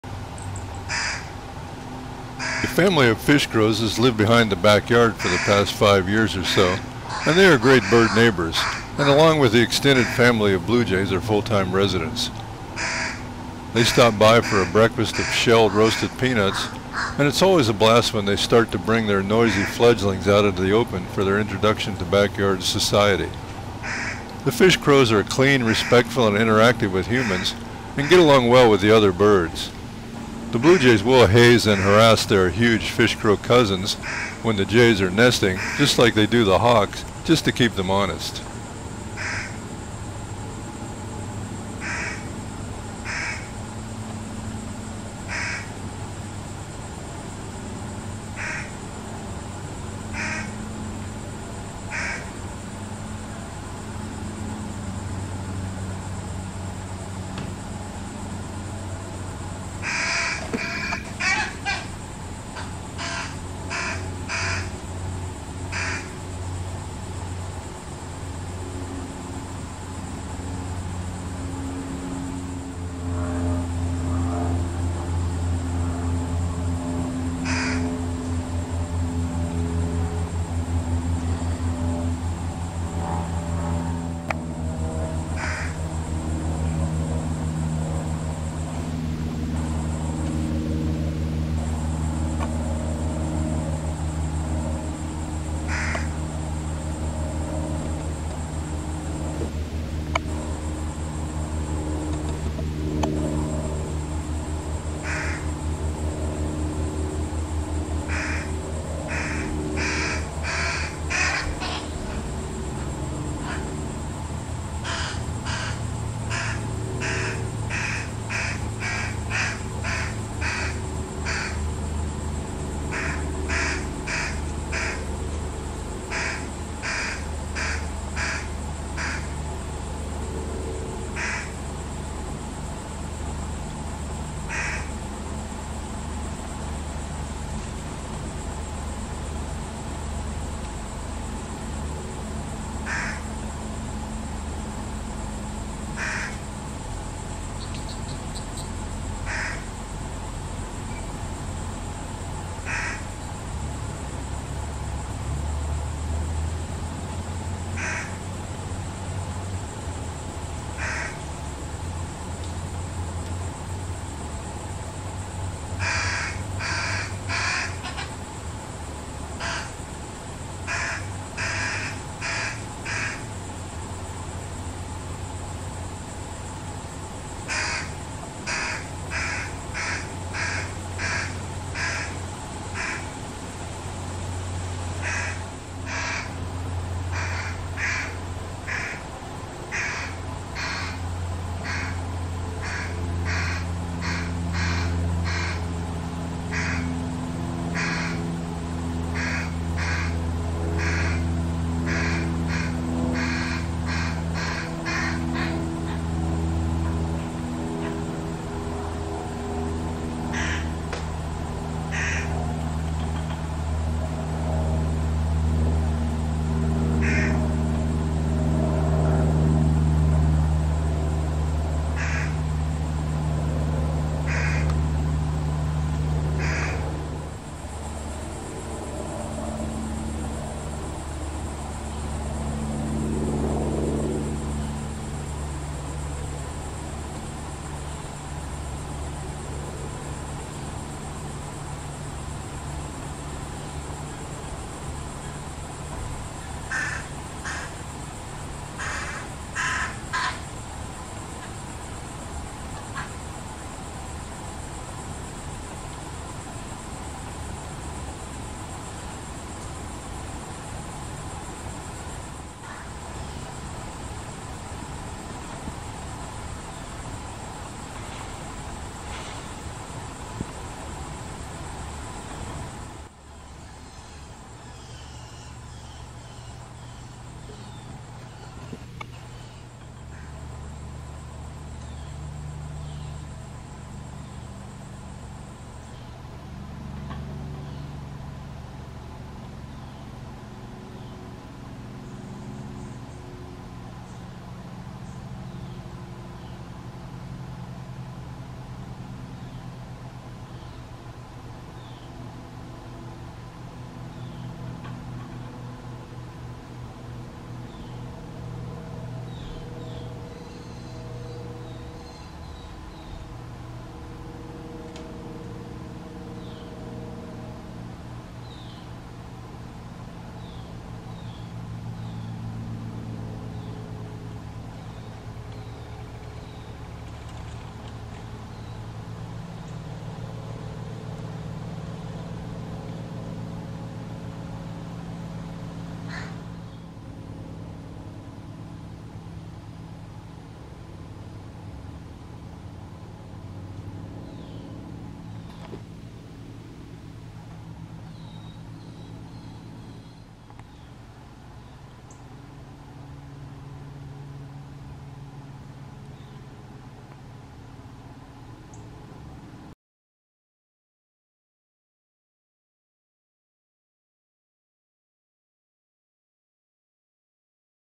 Fish Crow Fledglings Fed By Parents
A Fish Crow family has lived behind the Backyard for the past 5 years or so and they are great bird neighbors and along with the extended family of Blue Jays are full-time residents. They stop by for a breakfast of shelled roasted peanuts and its always a blast when they start to bring their noisy fledglings out into the open for their introduction to Backyard society.